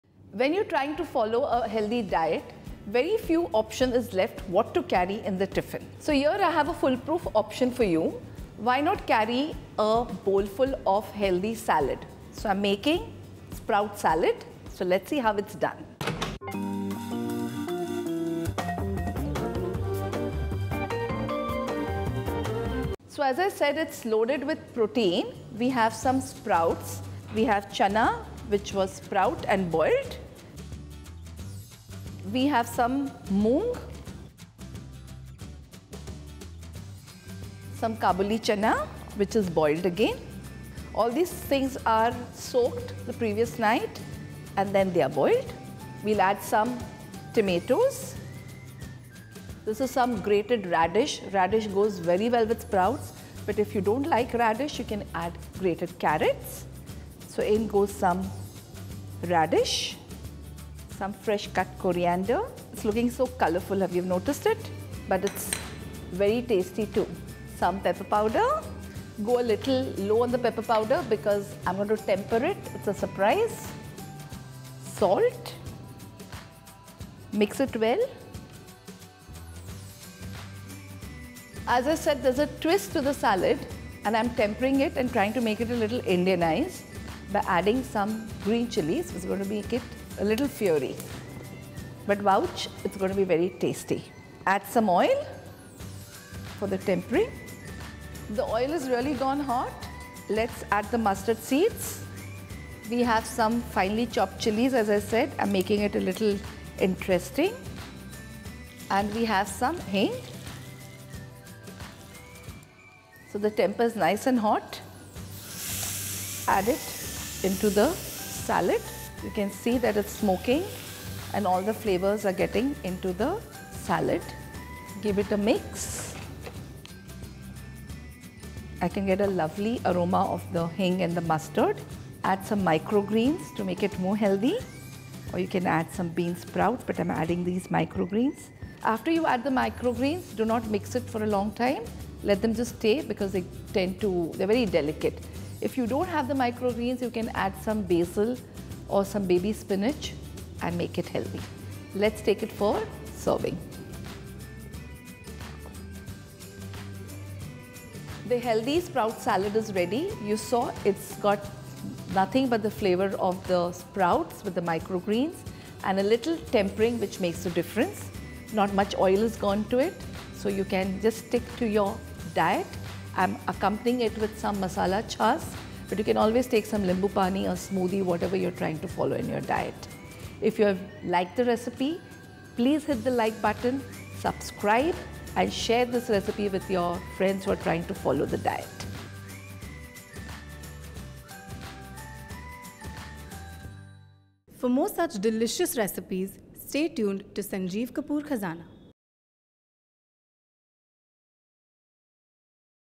Sprout Salad | Tiffin Treats by Roopa Nabar | Sanjeev Kapoor Khazana
Mixed sprouts tossed in Indian flavours and served with masala chaas.

SPROUT SALAD

Ingredients

1 cup chana sprouts, boiled
1 cup moong sprouts, boiled
½ cup chickpeas, soaked overnight and boiled
1 medium tomato, finely chopped
1 medium radish, grated
1 tablespoon chopped fresh coriander leaves
1 tablespoon oil
1 teaspoon mustard seeds
1-2 green chillies, finely chopped
¼ teaspoon asafoetida
¼ cup amaranth micro greens
Masala chaas for serving

Method

1. Take chana sprouts in a bowl. Add moong sprouts, chickpeas, tomatoes, radish, coriander leaves, crushed peppercorns and salt and mix well.
2. To prepare tempering, heat oil in a non-stick tempering pan. Add mustard seeds and let it splutter.
3. Add green chillies and asafoetida. Add the tempering to the salad mixture and mix well.
4. Add micro greens and mix lightly.
5. Serve with masala chaas.